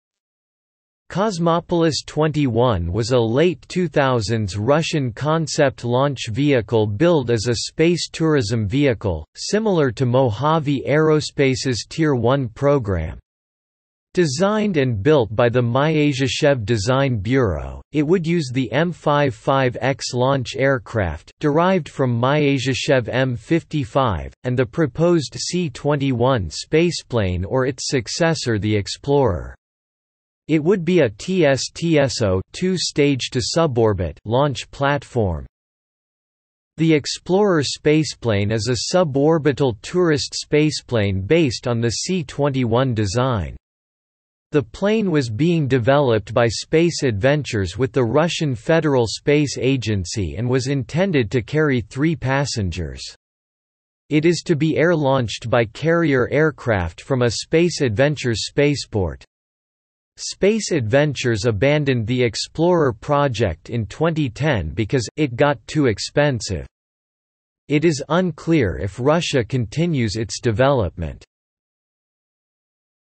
Space Adventures M-55X | Wikipedia audio article
- Socrates

SUMMARY
Cosmopolis XXI was a late 2000s Russian concept launch vehicle billed as a space tourism vehicle, similar to Mojave Aerospace's Tier One program. Designed and built by the Myasishchev Design Bureau, it would use the M-55X launch aircraft (derived from Myasishchev M-55), and the proposed C-21 spaceplane or its successor the Explorer. It would be a TSTSO (Two-Stage to SubOrbit) launch platform.
The Explorer spaceplane is a suborbital tourist spaceplane based on the  C-21 design. The plane was being developed by Space Adventures with the Russian Federal Space Agency and was intended to carry 3 passengers. It is to be air-launched by carrier aircraft from a Space Adventures spaceport.  Space Adventures abandoned the Explorer project in 2010 because "it got too expensive."  It is unclear if Russia continues its development.